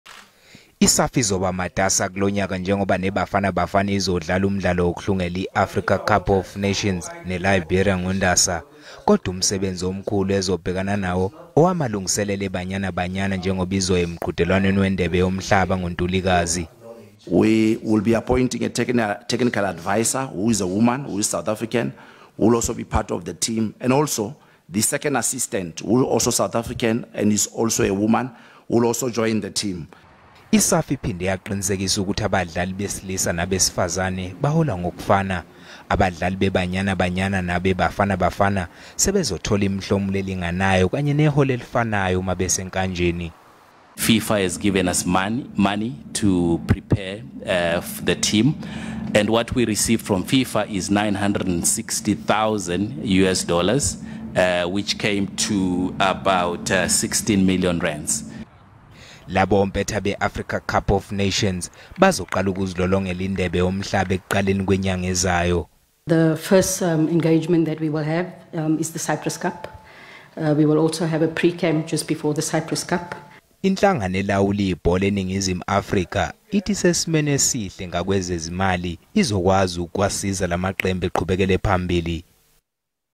Kuzoqiniswa ithimba labaqeqeshi le Banyana Banyana
Ithimba labaqeqeshi be-Banyana Banyana lizohlangana ngaphambi komqhudelwano weNdebe Yomhlaba yabesifazane ozosingathelwa e-Australia naseNew Zealand kiwo lonyaka. Leliqembu labesifazane lakuleli, lizoba nomqondisi wethimba labaqeqeshi kanye nomsizi wesibili kaMqeqeshi ngaphambi kwalomqhudelwano ozoqala mhla zingamashumi amabili kuNtulikazi.